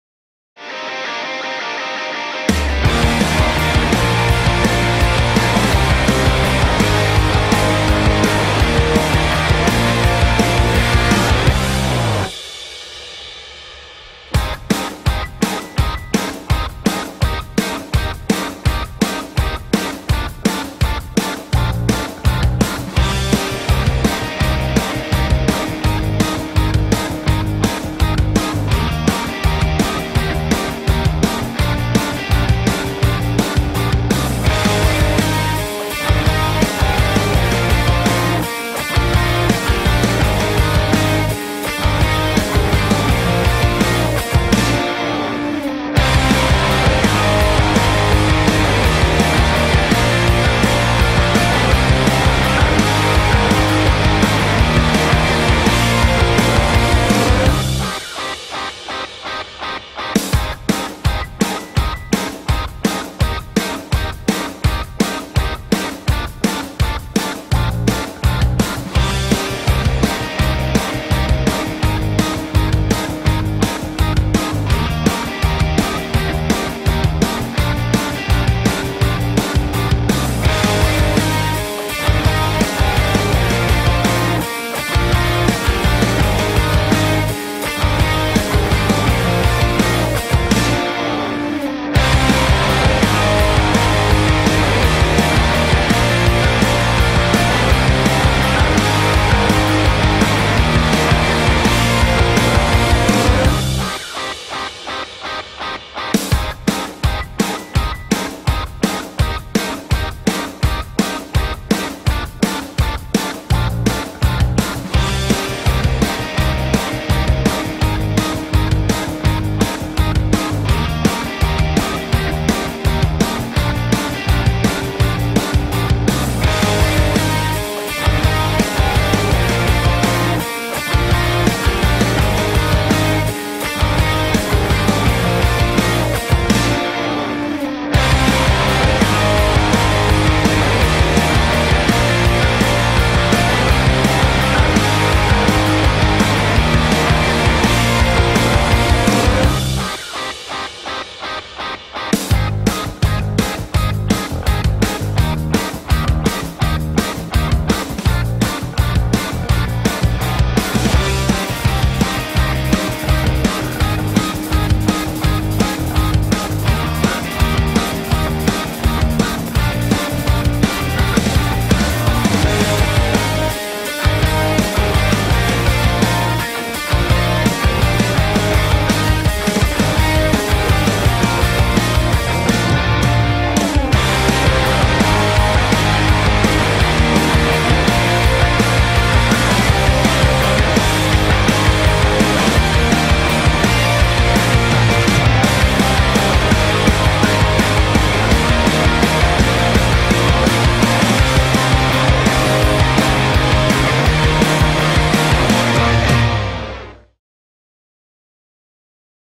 Skydance Pedestrian Bridge - Oklahoma City, Oklahoma
Manhattan Road & Bridge constructed the iconic Skydance Pedestrian Bridge located in Oklahoma City, Oklahoma over Interstate 40. The design is intended to represent Oklahoma's state bird, the Scissor-tailed Flycatcher.